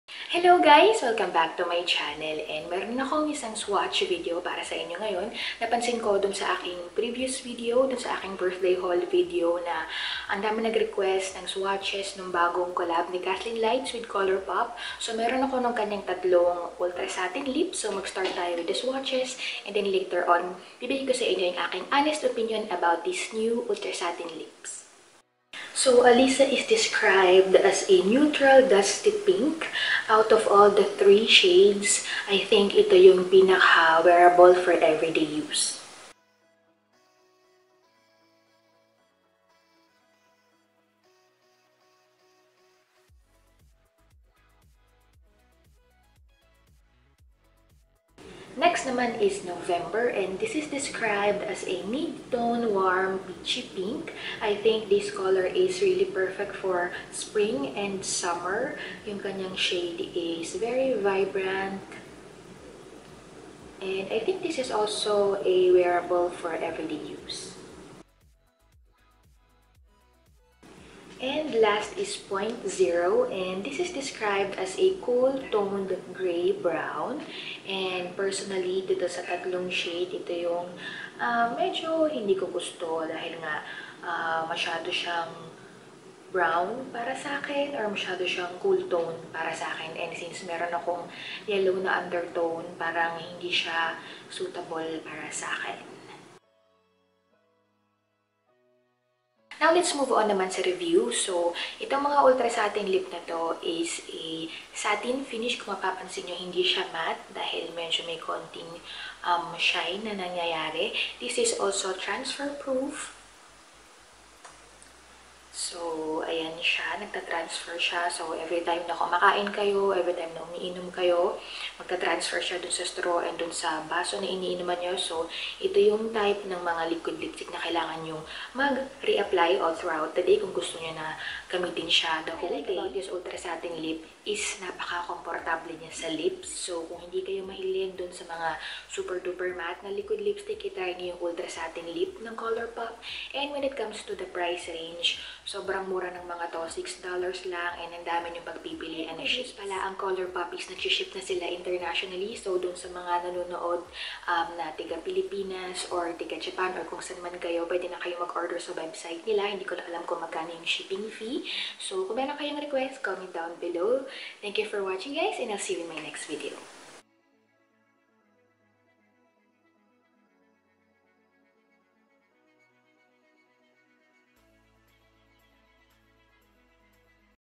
Kathleen Lights x ColourPop Collection Review and Swatches| TAGALOG
Kathleen Lights x ColourPop Collection Review and Swatches

Again this is NOT transfer proof! nalito lang ang lola niyo, hahaha! :D

Alyssa
November
Point Zero

► Please THUMBS UP if you liked the video =)

ROAD TRIP | MT. RUSHMORE | BADLANDS

❋ ❋ Follow Me ❋ ❋

username: mrs_noche